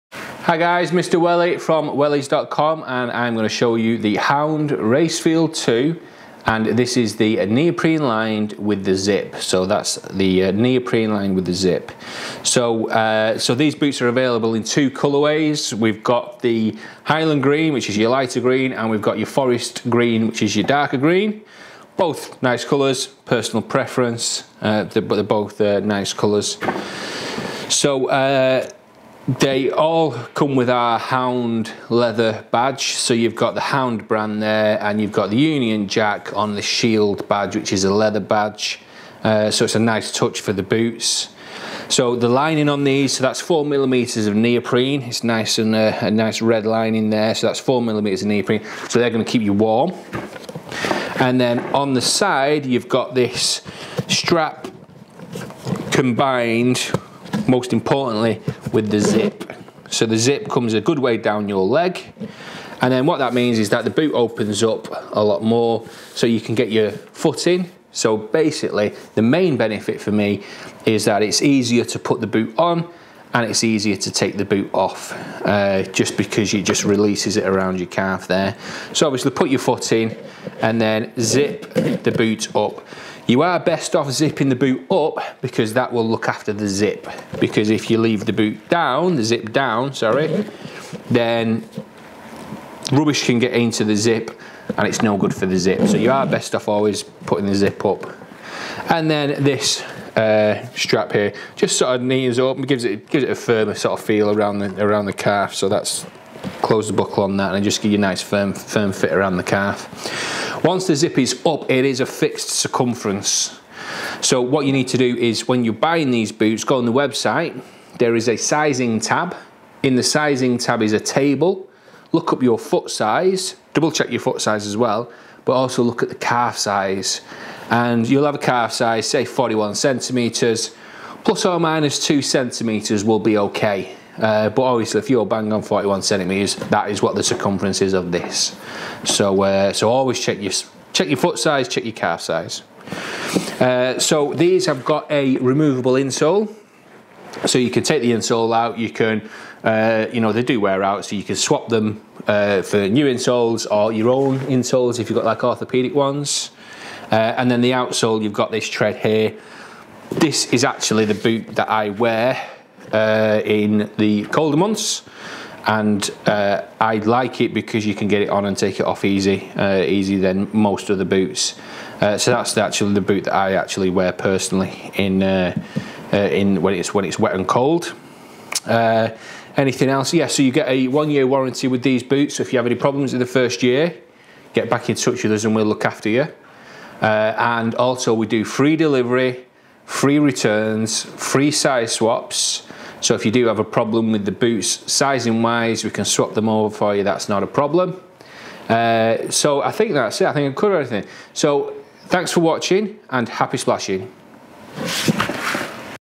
Hound Racefield 2 Neoprene Zip
Mr Welly from Wellies.com talks about Hound Racefield 2 Wellies with a Neoprene Lining and Zip.